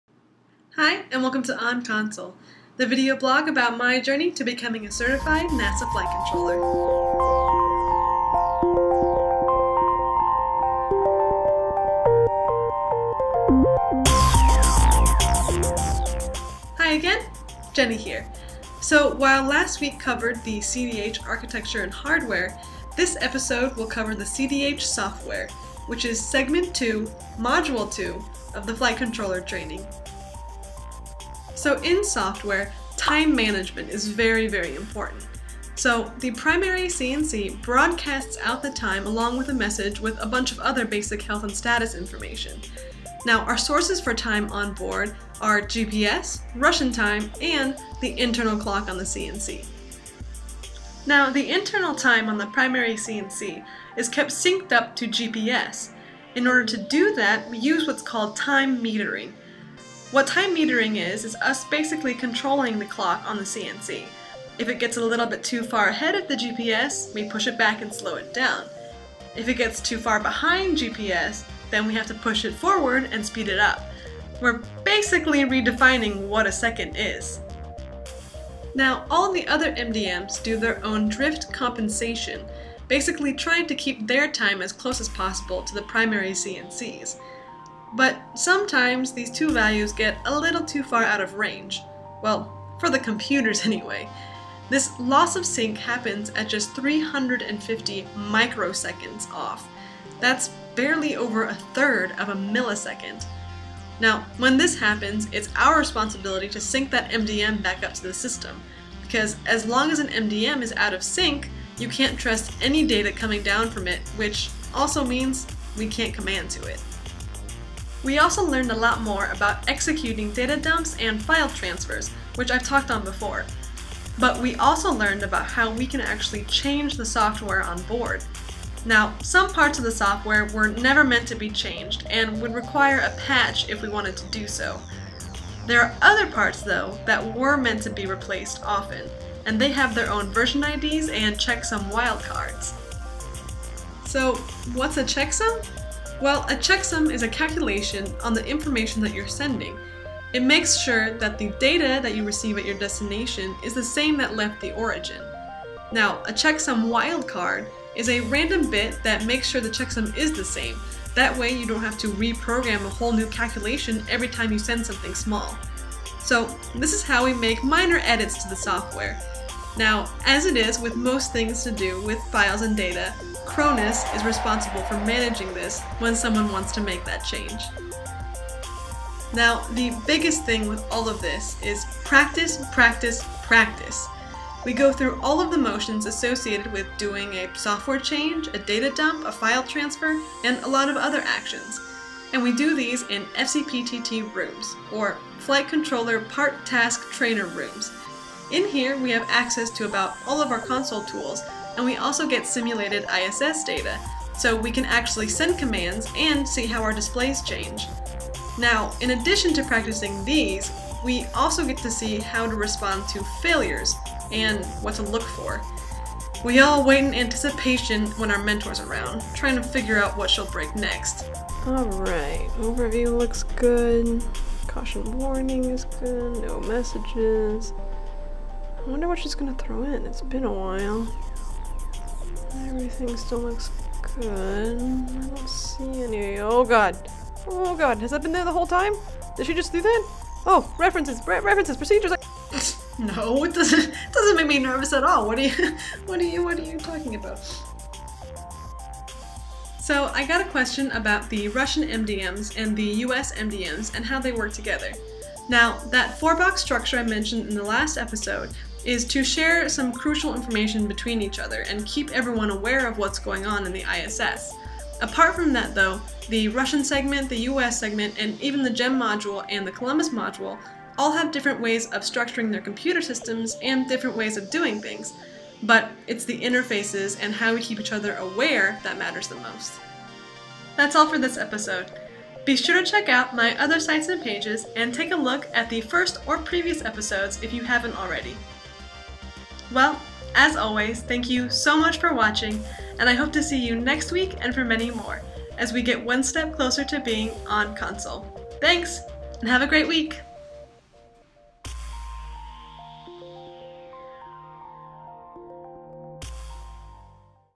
Jenny On Console: Episode 9 - ISS Computer Software
Hi and welcome to Episode 9 of Jenny on Console! Learn more about the second segment of the ISS Flight Controller Technical Flow. This time I talk about the software onboard the ISS, including controlling time!